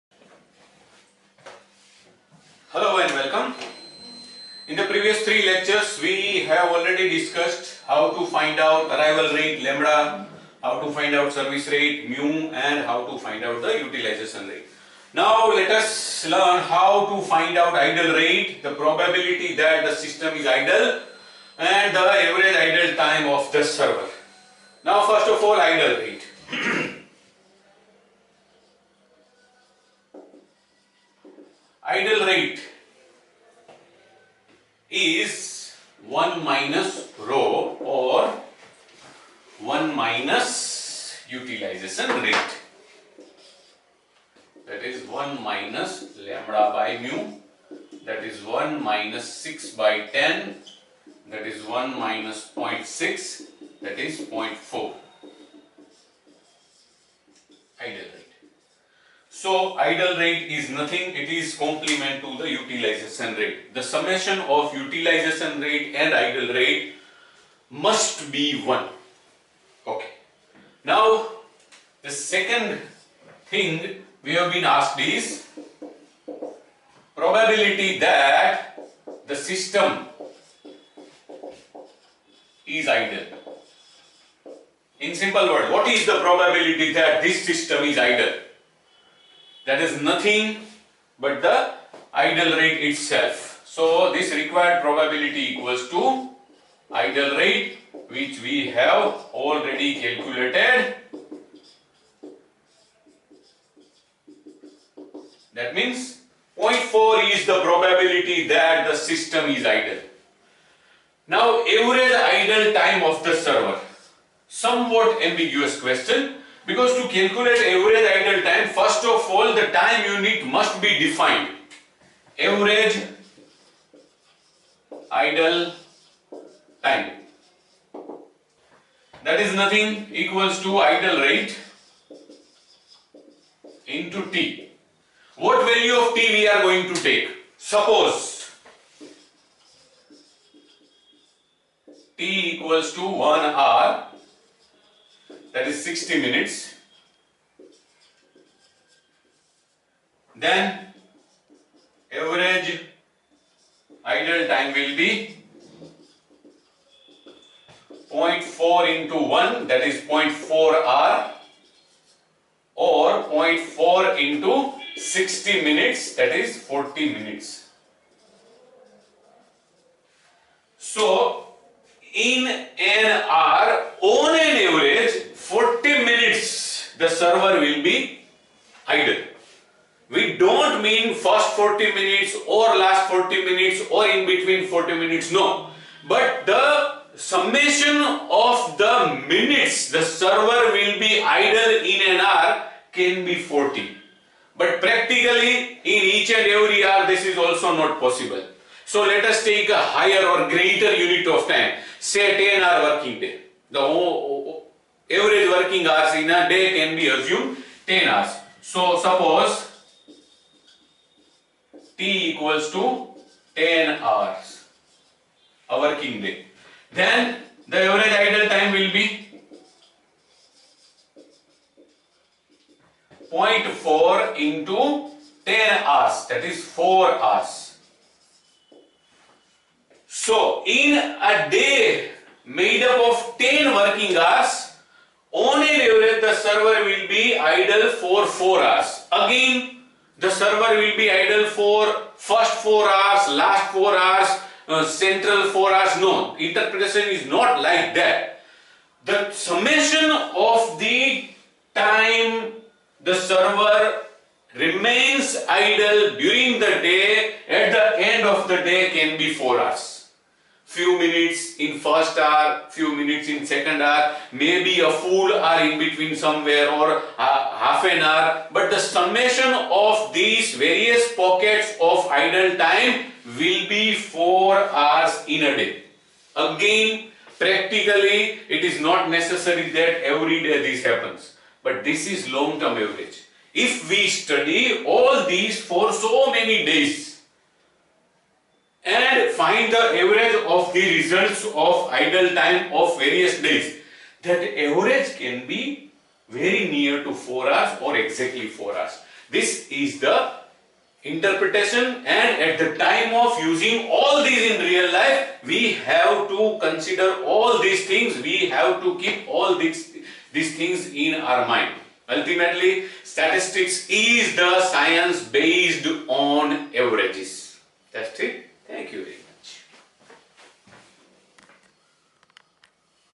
Queuing Theory - 13 Single Server Model Calculating Idle Rate and Average Idle Time
Operations Management - OM
Queuing Theory
Queuing Models
Single Server Model
Idle Rate

Poisson - Exponential Single Server Model

Case:
On the help desk of an office of an insurance company, the arrivals of the customers follow the Poisson Law and, on an average, at every 10 minutes a customer arrive. The officer responsible takes, on an average, 6 minutes to serve a customer, assuming the service exponentially distributed.
* Find (i) dle Rate (ii) Prob(No customer is in the system) (iii) Average Idle Time for 't' = 1 hour
(i)  Idle Rate = 1 - ρ = (1 - λ/μ), the system is idle, that means there us no customer in the system.
(i) Time unit of an hour (i.e. 60 minutes)
* Idle Rate = 1 - ρ = (1 - λ/μ)
= 1 (6/10)
= 1 - 0.6
= 0.4
(ii) Prob (No customer in the syste) = Idle Rate = 1 - ρ = (1 - λ/μ) = 0.4
(iii) Average Idle Time for 't' = 1 hour = 60 minuts
Average Idle Time = Idle Rate x 't'
= 0.4 x 1 hour = 0.4 hour
= 0.4 x 60 minutes = 24 minutes. (THIS HAS BEEN WRONGLY WRITTEN AS 40 MINUTES ON BOARD IN THE VIDEO. I AM VERY SORRY FOR THAT WRITING MISTAKE)

Average Idle Time for a 10-hour working day = 0.4 x 10 hours = 4 hours per working day